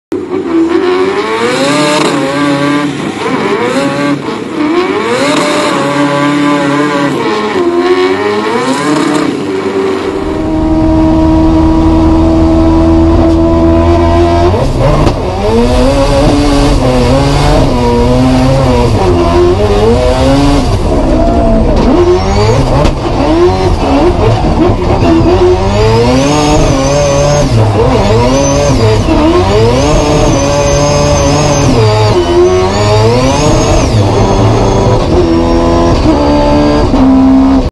Quad rotor Lexus gs300 with a dogbox
Dog box drifting from a 4 rotor lexus gs300
Widebody Mazda motor rx7 mash up
Brap
Drift build